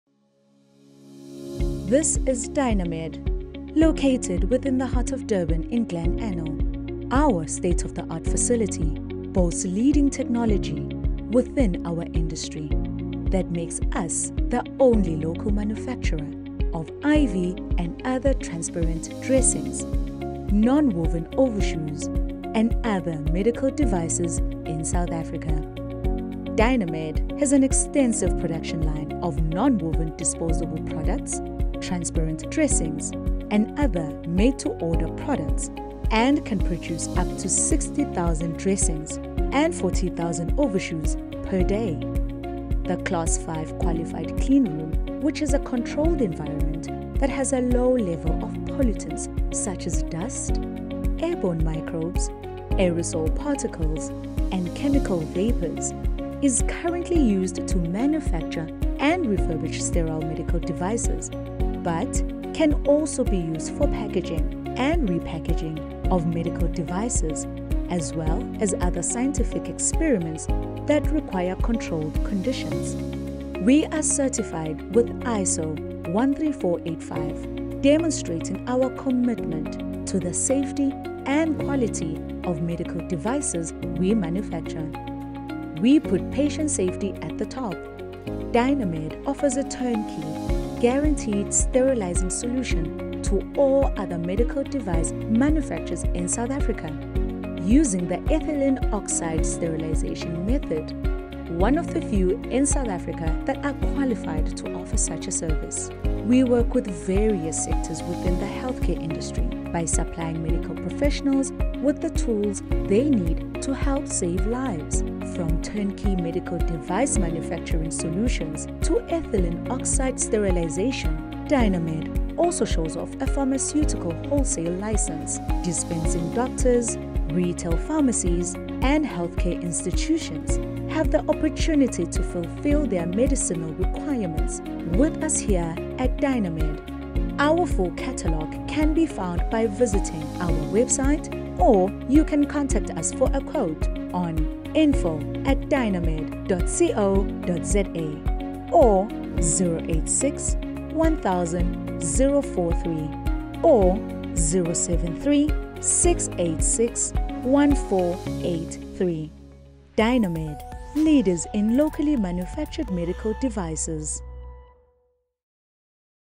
Dynamed Introduction
This is Dynamed. Located within the heart of Durban in Glen Anil.

Our state of the art facility boasts leading technology within our industry that makes us the only local manufacturer of IV and other transparent dressings, non-woven overshoes and other medical devices in South Africa.

Dynamed has an extensive production line of non-woven disposable products, transparent dressings and other made-to-order products, and can produce up to 60 thousand dressings and 40 thousand overshoes per day.

The class 5 qualified cleanroom, which is a controlled environment that has a low level of pollutants such as dust, airborne microbes, aerosol particles, and chemical vapors, is currently used to manufacture and refurbish sterile medical devices but, can also be used for packaging and repackaging of medical devices, as well as other scientific experiments that require controlled conditions.

We are certified with ISO 13485 demonstrating our commitment to the safety and quality of medical devices we manufacture. We put patient safety at the top.

Dynamed offers a turnkey, guaranteed sterilizing solution to all other medical device manufacturers in South Africa using the Ethylene Oxide sterilization method; One of the few in South Africa that are qualified to offer such a service.

We work with various sectors within the healthcare industry by supplying medical professionals with the tools they need to help save lives. From turnkey medical device manufacturing solutions to Ethylene Oxide sterilization, Dynamed also shows off a pharmaceutical wholesale license. Dispensing doctors, retail pharmacies and healthcare institutions have the opportunity to fulfil their medicinal requirements with us here at Dynamed.

Dynamed. Leaders in locally manufactured medical devices